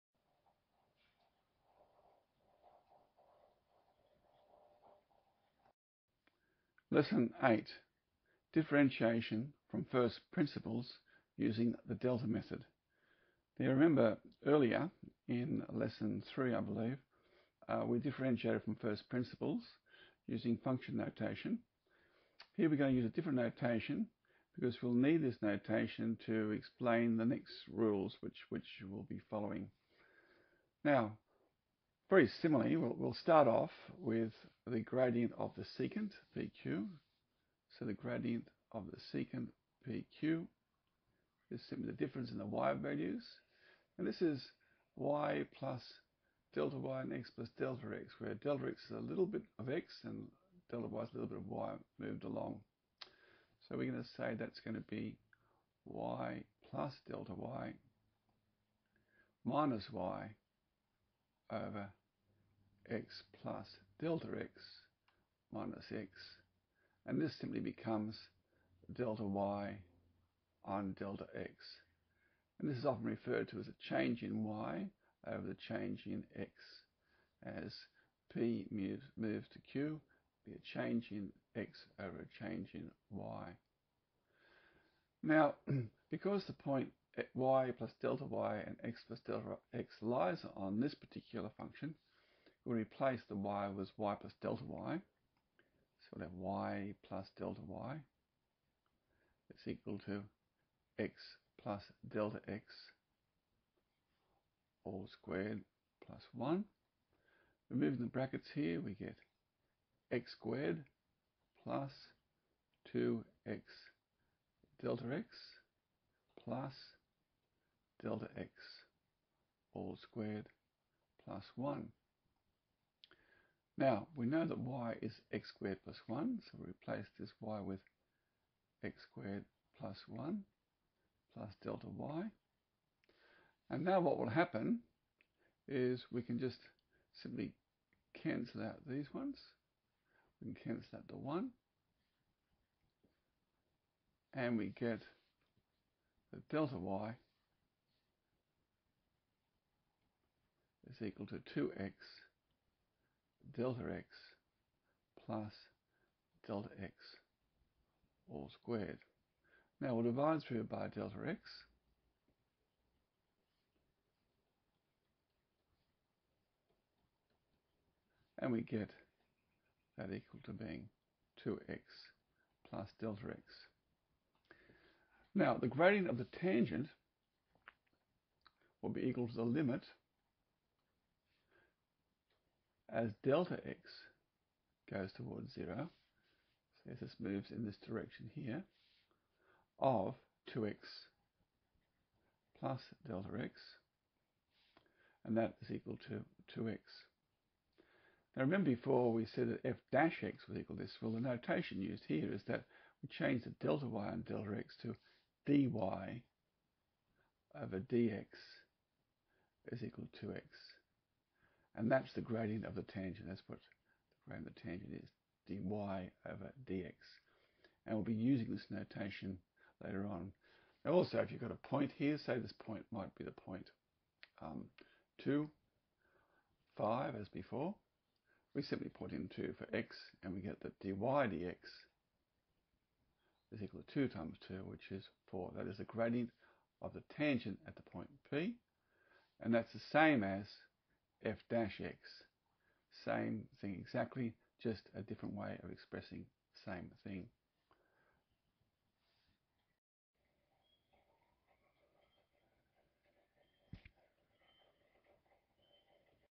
Calculus 1 Lesson 8: Differentiation using the Delta Method
Differentiation from First Principles using the Delta Method is explained.The dy/dx notation is introduced.